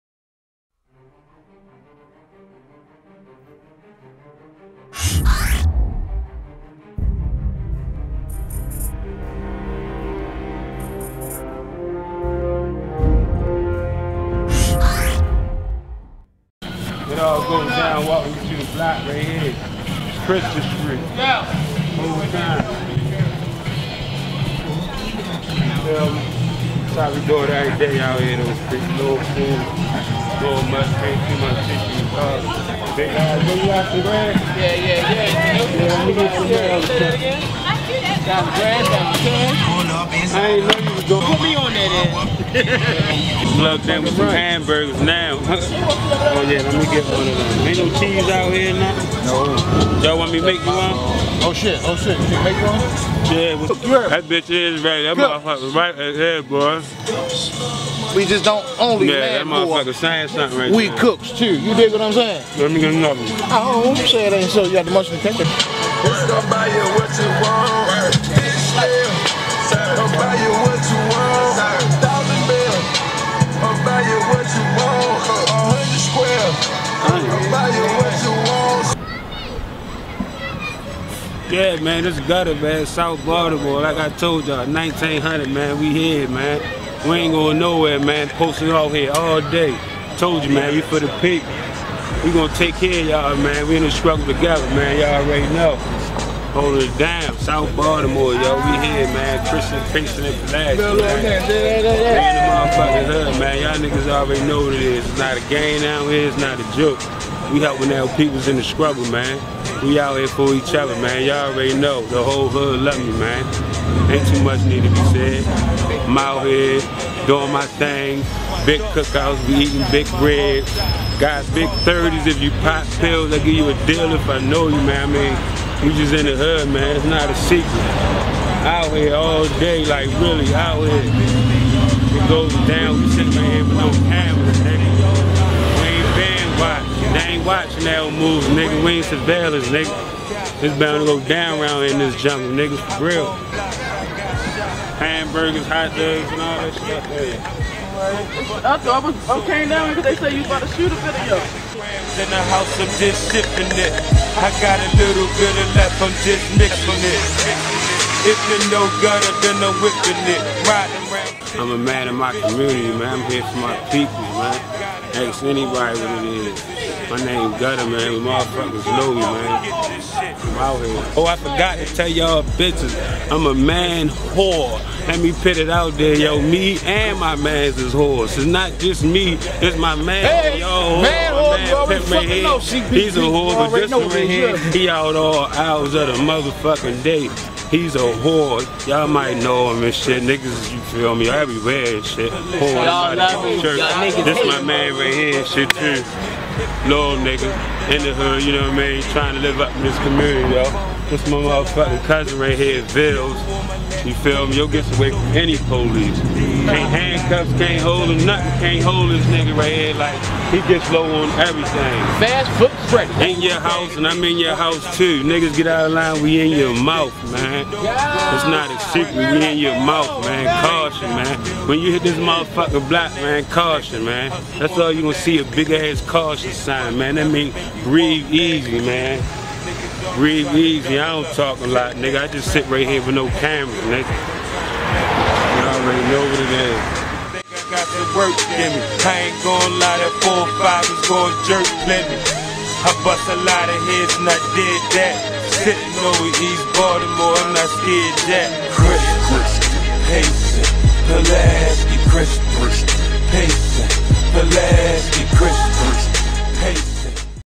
(((THE PLUG))) VLOG - GUTTA LOVE'S (CHRISTIAN ST. COOK OUT)
(((THE PLUG))) PROMO - GUTTA LOVE'S (CHRISTIAN ST. COOK OUT)

Up and coming local DMV artist Gutta Love invites "The Plug" to his weekly Christian St. cook out. Everyone was welcomed!!! DONT MISS NEXT WEEK COOK OUT!!! FREE FOOD!!!